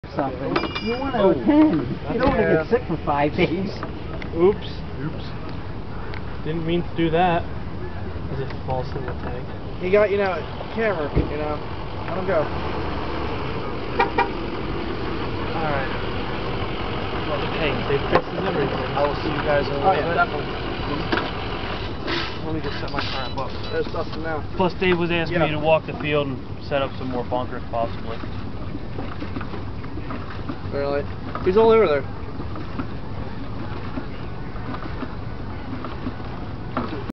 decker's bilge pump
decker filling up the "flame throwers" for the corn maze game